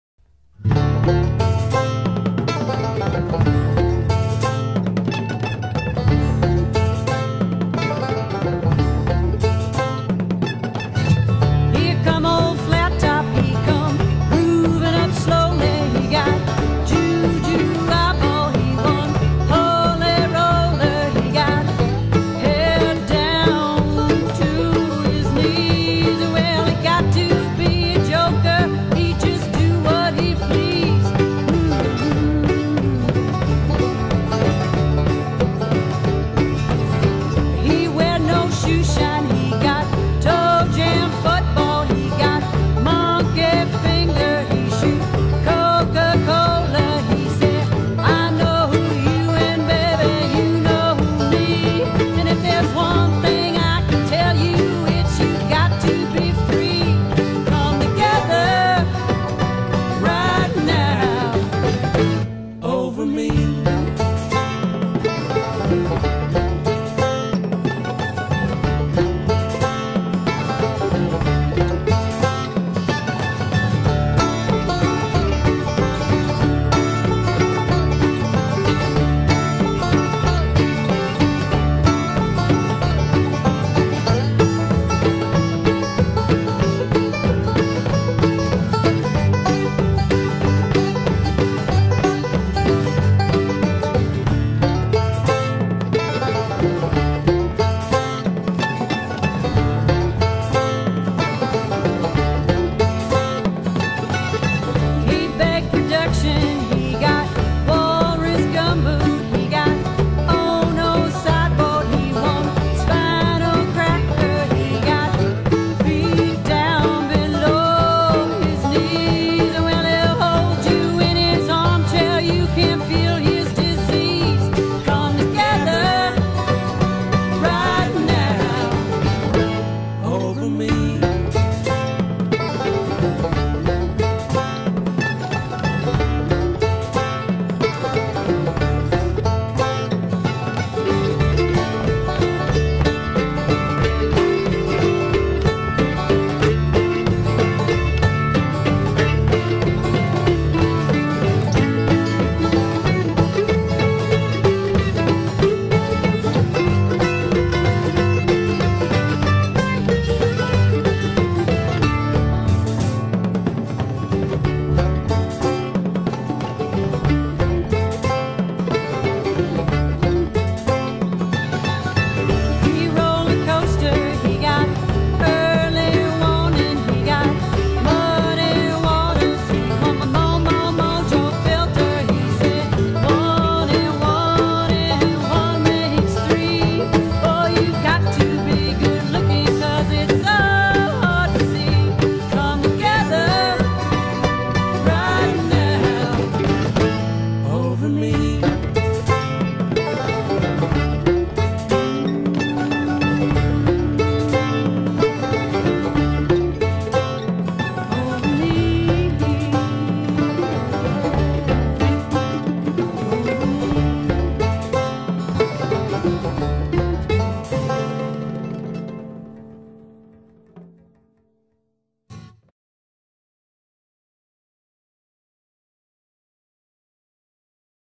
fish heads bluegrass band
this band rocks some of the old classics...done new grassy.